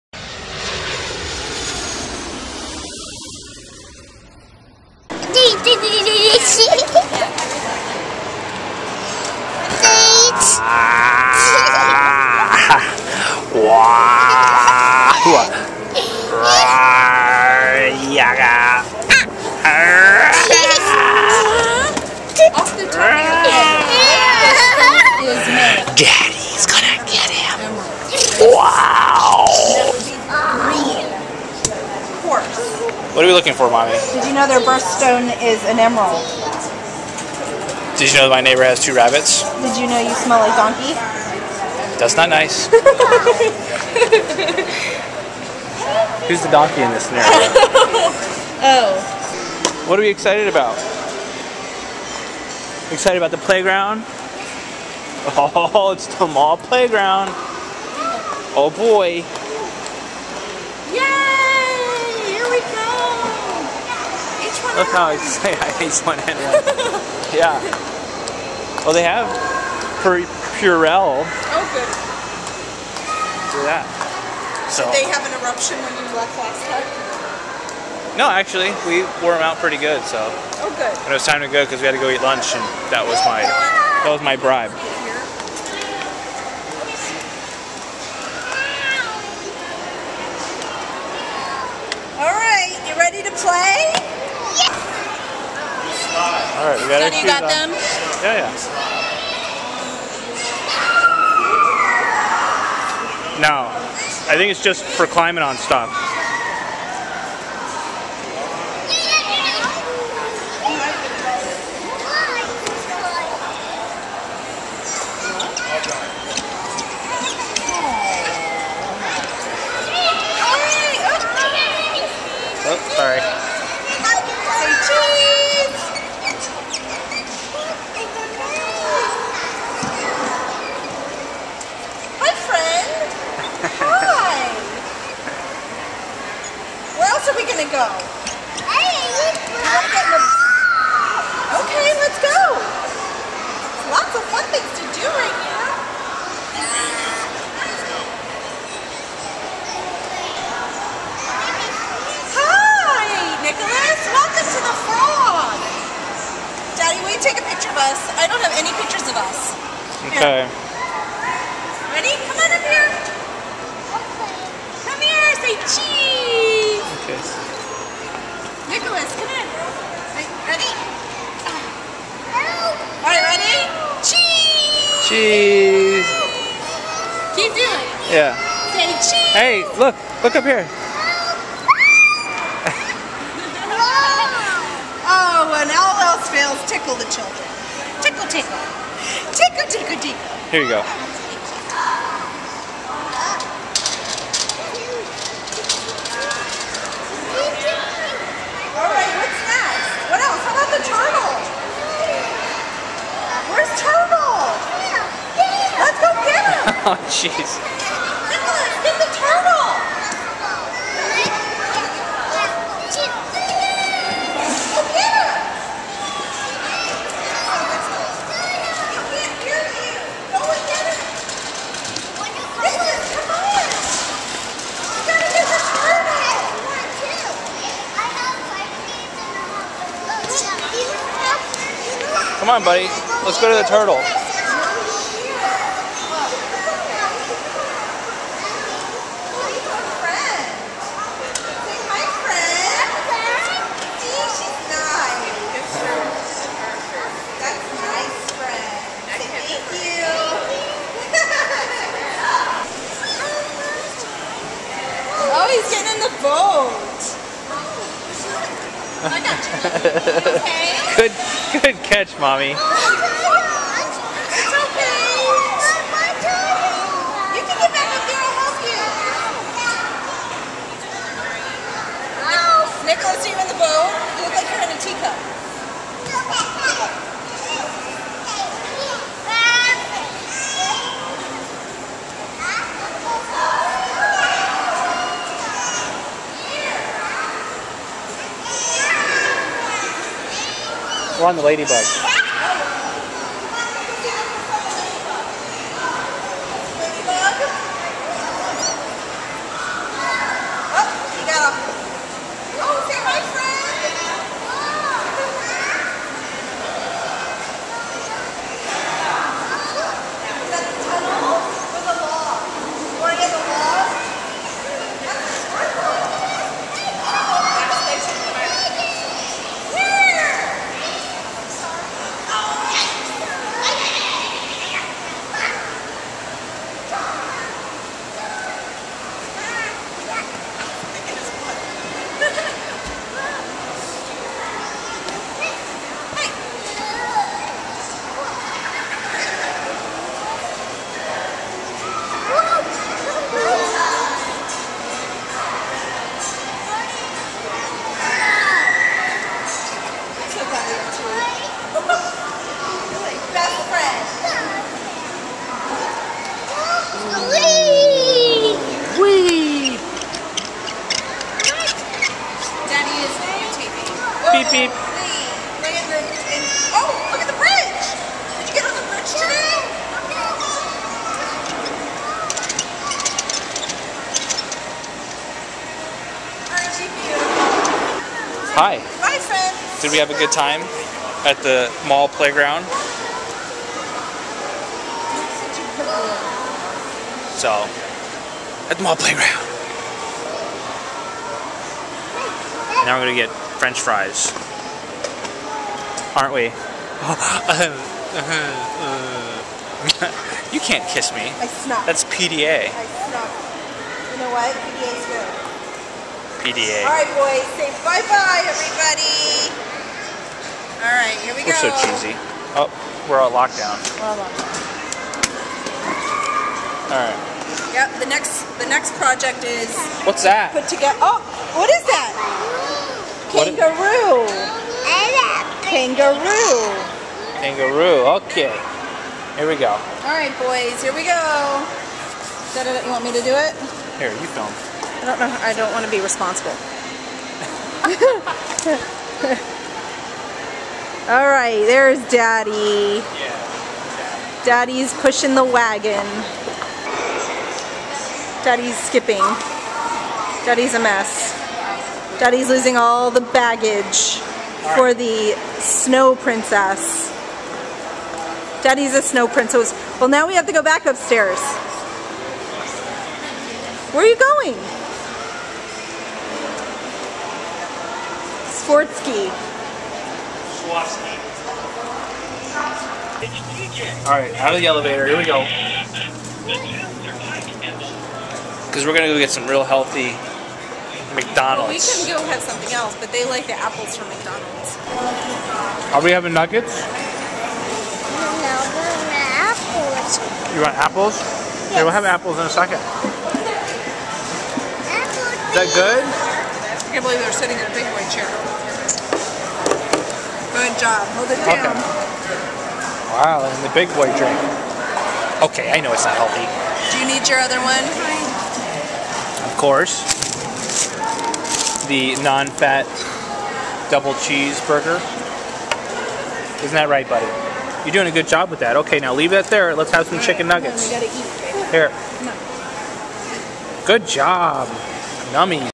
BOYS DRIVING CARS IN MALL!!
After we visited Santa Clause, we went to the mall playground. Good Times!